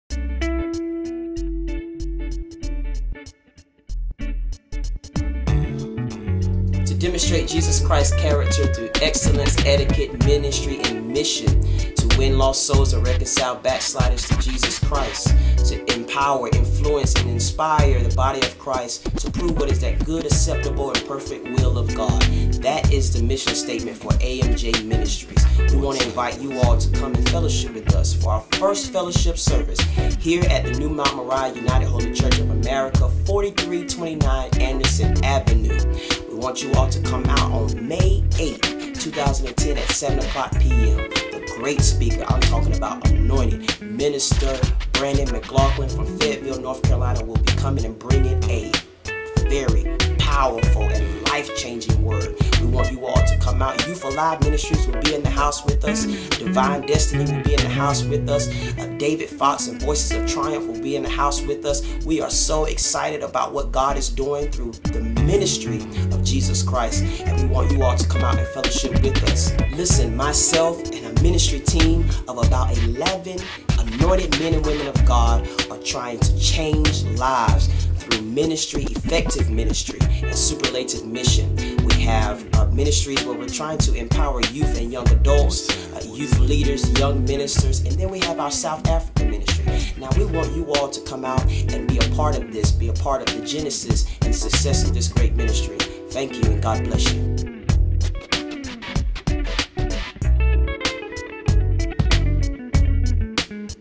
AMJ Ministries Fellowship
May 8, 2010 @ New Mt. Moriah Church
Guest Minister: Brandon McLauchlin
Service Starts @ 7 p.m.